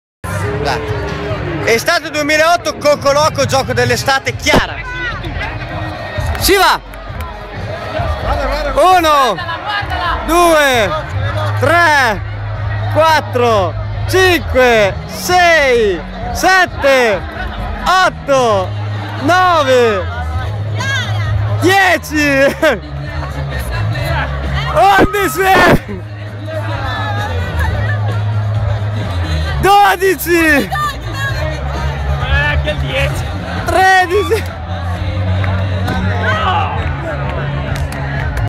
Gioco dell'estate 2008 - CHIARA
20 giri su sestesso, fissando una moneta e poi buttarla a te